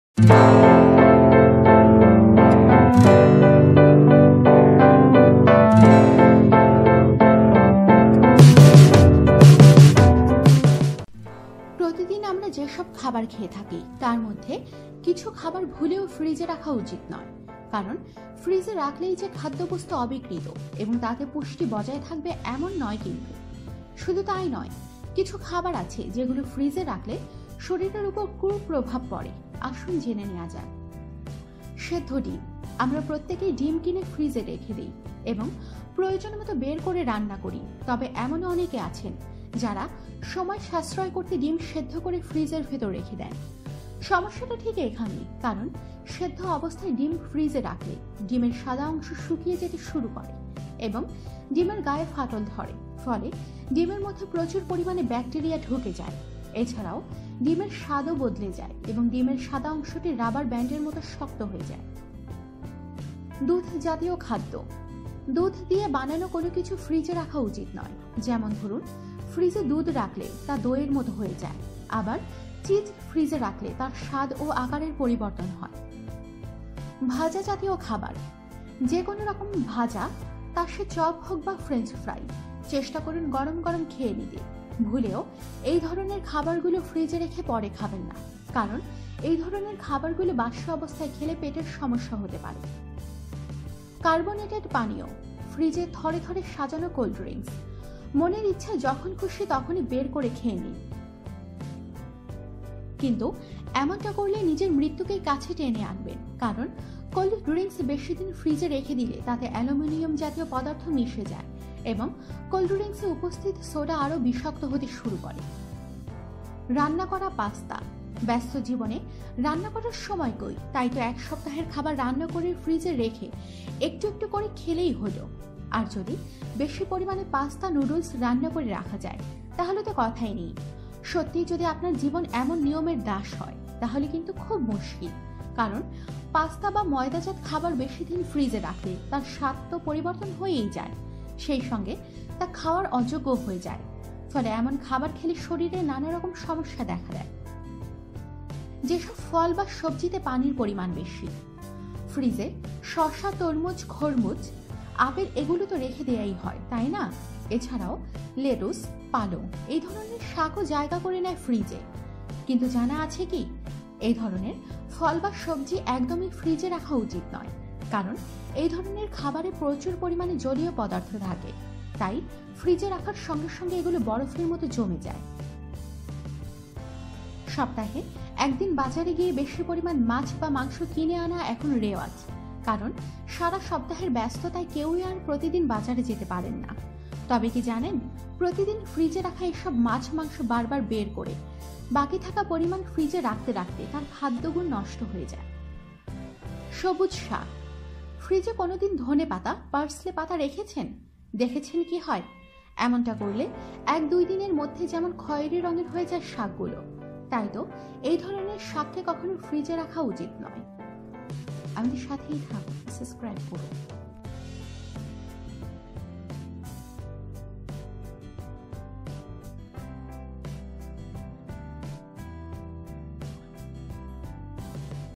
ভুলেও ফ্রিজে রাখবেন না এই খাবারগুলি!
প্রতিদিন আমরা যেসব খাবার খেয়ে থাকি, তার মধ্যে কিছু খাবার ভুলেও ফ্রিজে রাখা উচিত নয়। কারণ ফ্রিজে রাখলেই যে খাদ্যবস্তু অবিকৃত এবং তাতে পুষ্টি বজায় থাকবে, এমন নয় কিন্তু! শুধু তাই নয়, কিছু খাবার আছে যেগুলি ফ্রিজে রাখলে শরীরের ওপর কু-প্রভাব পড়ে। আসুন জেনে নেওয়া যাক...

১. সেদ্ধ ডিম
আমরা প্রত্যেকেই ডিম কিনে ফ্রিজে রেখে দিই এবং প্রয়োজন মতো বের করে রান্না করি। তবে এমনও অনেকে আছেন, যারা সময় সাশ্রয় করতে ডিম সেদ্ধ করে ফ্রিজের ভেতরে রেখে দেন। সমস্যাটা ঠিক এখানেই। কারণ সেদ্ধ অবস্থায় ডিম ফ্রিজে রাখলে ডিমের সাদা অংশ শুকিয়ে যেতে শুরু করে এবং ডিমের গায়ে ফাটল ধরে। ফলে ডিমের মধ্যে প্রচুর পরিমাণে ব্যাকটেরিয়া ঢুকে যায়। এ ছাড়াও ডিমের স্বাদও বদলে যায় এবং ডিমের সাদা অংশটি রাবার ব্যান্ডের মতো শক্ত হয়ে যায়। তাই শরীরকে সংক্রমণের হাত থেকে বাঁচাতে ফ্রিজে সেদ্ধ ডিম রাখা চলবে না।

২. দুধজাত খাদ্য
দুধ দিয়ে বানানো কোনো কিছুই ফ্রিজে রাখা উচিত নয়। যেমন ধরুন, ফ্রিজে দুধ রাখলে তা দইয়ের মতো হয়ে যায়।

আবার, চিজ ফ্রিজে রাখলে তারও স্বাদ এবং আকারের পরিবর্তন হয়।
৩. ভাজাজাতীয় খাবার
যেকোনোরকম ভাজা, তা সে চপ হোক বা ফ্রেঞ্চফ্রাই, চেষ্টা করুন গরম গরম খেয়ে নিতে। ভুলেও এই ধরনের খাবারগুলি ফ্রিজে রেখে পরে খাবেন না। কারণ এই ধরণের খাবারগুলো বাসি অবস্থায় খেলে পেটের সমস্যা হতে পারে। এ ছাড়াও হৃদরোগের সম্ভাবনা বাড়ে।

৪. কার্বোনেটেড পানীয়
ফ্রিজে থরে থরে সাজানো কোল্ড ড্রিঙ্কস। মনের ইচ্ছায় যখন খুশি পান করলেই হলো। কিন্তু এমনটা করলে নিজের মৃত্যুকেই কাছে টেনে আনবেন। কারণ কোল্ড ড্রিঙ্কস বেশিদিন ফ্রিজে রেখে দিলে তাতে অ্যালুমিনিয়াম জাতীয় পদার্থ মিশে যায় এবং কোল্ড ড্রিঙ্কসে উপস্থিত সোডা আরও বিষাক্ত হতে শুরু করে। তাই কোল্ড ড্রিঙ্কস পান করার ইচ্ছা হলে কেনার পরপরই খেয়ে নেবেন। বেশিদিন ফ্রিজে রেখে পান করবেন না।

৫. রান্না করা পাস্তা
ব্যস্ত জীবনে রান্না করার সময় কই! তাইতো এক সপ্তাহের খাবার রান্না করে ফ্রিজে রেখে একটু একটু করে খেলেই হলো। আর যদি বেশি পরিমাণে পাস্তা, নুডলস রান্না করে রাখা যায়, তাহলে তো কথাই নেই। সত্যিই যদি আপনার জীবন এমন নিয়মের দাস হয়, তাহলে কিন্তু খুব মুশকিল। কারণ পাস্তা বা ময়দাজাত খাবার বেশিদিন ফ্রিজে রাখলে তার স্বাদ তো পরিবর্তন হয়েই যায়, সেই সঙ্গে তা খাওয়ার অযোগ্যও হয়ে যায়। ফলে এমন খাবার খেলে শরীরের নানারকম সমস্যা দেখা দেয়। তাই কোনো ভাবেই পাস্তা জাতীয় খাবার বেশি দিন ফ্রিজে রাখা উচিত নয়।

৬. যে সব ফল বা সবজিতে পানির পরিমাণ বেশি
ফ্রিজে শসা, তরমুজ, খরমুজ, আপেল এগুলো তো রেখে দেওয়াই হয়, তাই না? এ ছাড়াও লেটুস, পালং এই ধরনের শাকও জায়গা করে নেয় ফ্রিজে। কিন্তু জানা আছে কি, এই ধরনের ফল বা সবজি একদমই ফ্রিজে রাখা উচিত নয়। কারণ এই ধরনের খাবারে প্রচুর পরিমাণে জলীয় পদার্থ থাকে। তাই ফ্রিজে রাখার সঙ্গে সঙ্গে এগুলি বরফের মতো জমে যায়। আবার ফ্রিজ থেকে বার করে আনলে এগুলো নরম হতে শুরু করে। ফলে স্বাদ একেবারে নষ্ট হয়ে যায়।

৭. মাছ বা মাংস
সপ্তাহে একদিন বাজারে গিয়ে বেশি পরিমাণ মাছ মাংস কিনে আনা এখন যেন রেওয়াজ। কারণ সারা সপ্তাহের ব্যস্ততায় কেউই আর প্রতিদিন বাজার যেতে পারেন না। তবে কী জানেন, প্রতিদিন ফ্রিজে রাখা এই সব মাছ মাংস বারবার বের করে, বাকি থাকা পরিমাণ ফ্রিজে রাখতে রাখতে তাঁর খাদ্যগুণ নষ্ট হয়ে যায়। আরও মারাত্মক বিষয় হলো এই পদ্ধতির মাধ্যমে মাছ বা মাংসে প্রচুর পরিমাণে ব্যাকটেরিয়া বাসা বাঁধতে শুরু করে। ফলে এমন ধরনের খাবার খেলে শরীর খারাপ হওয়া কেউ আটকাতে পারে না।

৮. সবুজ শাক
ফ্রিজে কোনো দিন ধনেপাতা, পার্সলে পাতা রেখেছেন? দেখেছেন কি হয়? এমনটা করলে এক দুই দিনের মধ্যে কেমন খয়েরি রংয়ের হয়ে যায় শাকগুলি। তাই তো এই ধরনের শাককে কখনো ফ্রিজে রাখা উচিত নয়।